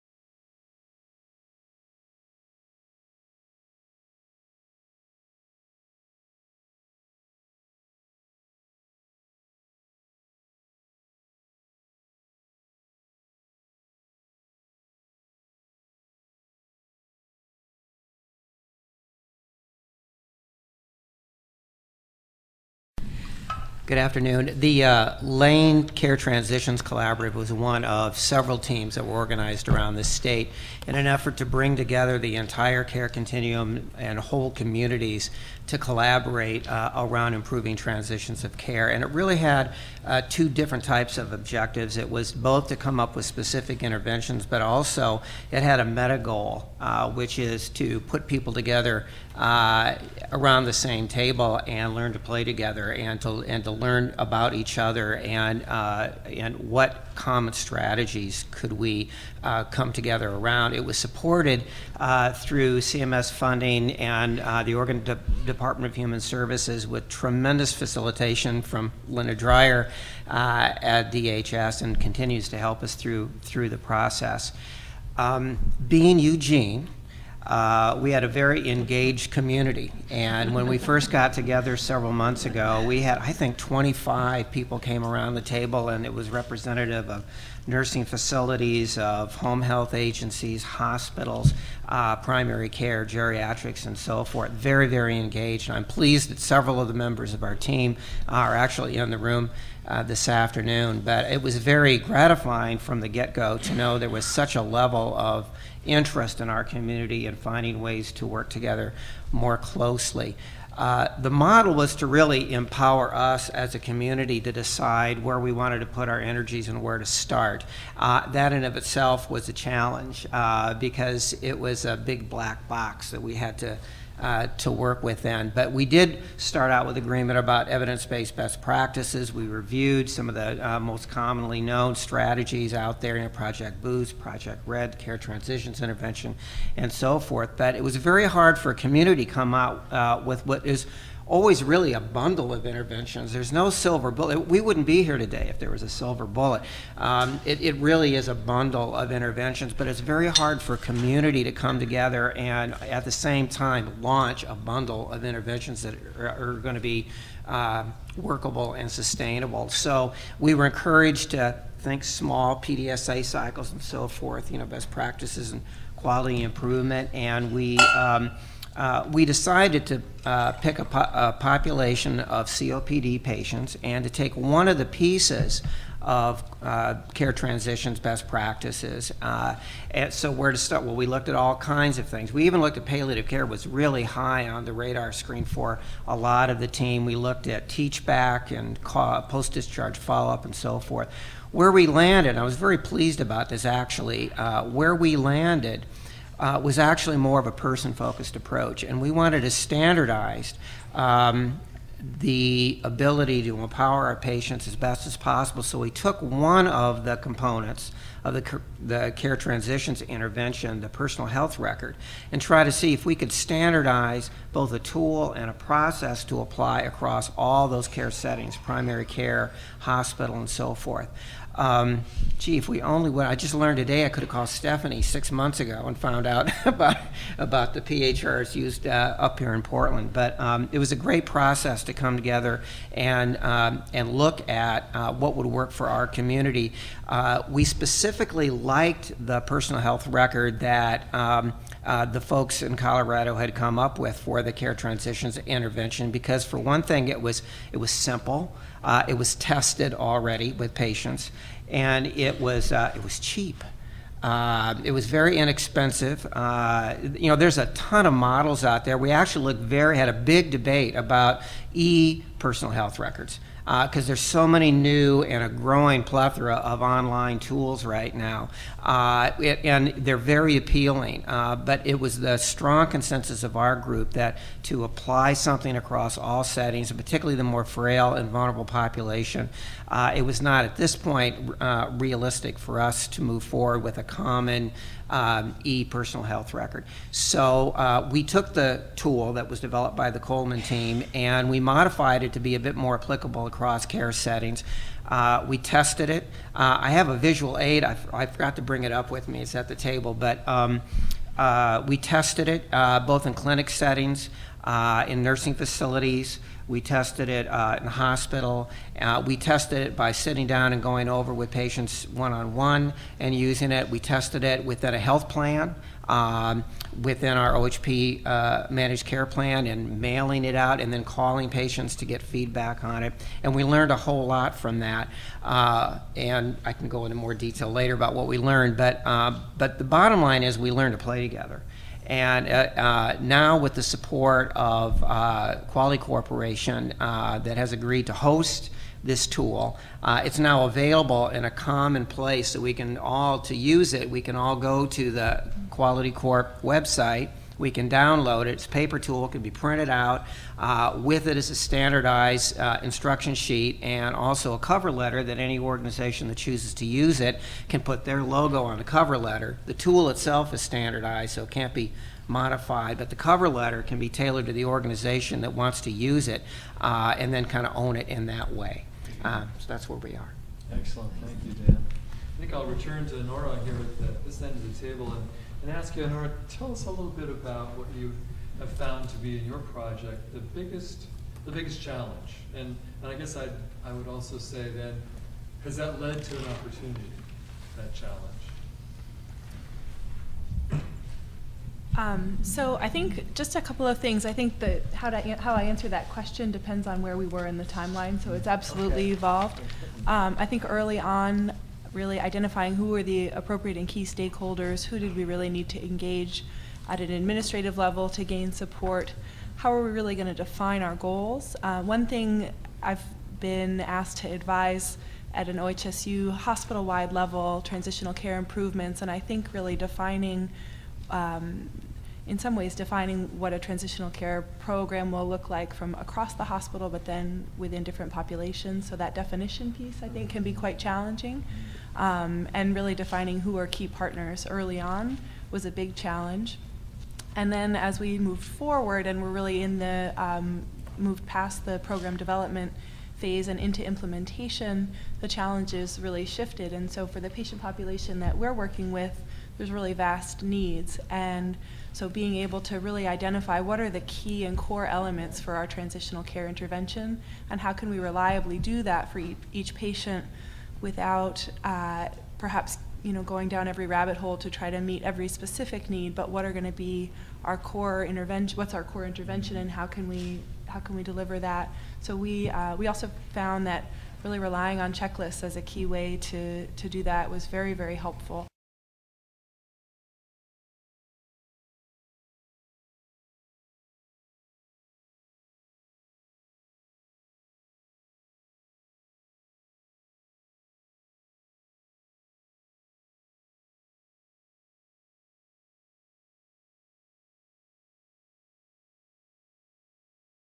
Oregon Projects Panel Part 3
On October 4, 2011, the Oregon Health Care Quality Corporation held its Reducing Readmissions in Oregon conference. The Oregon Project panel provided examples of activities in Oregon aimed at improving transitions in care and reducing unnecessary hospital readmissions.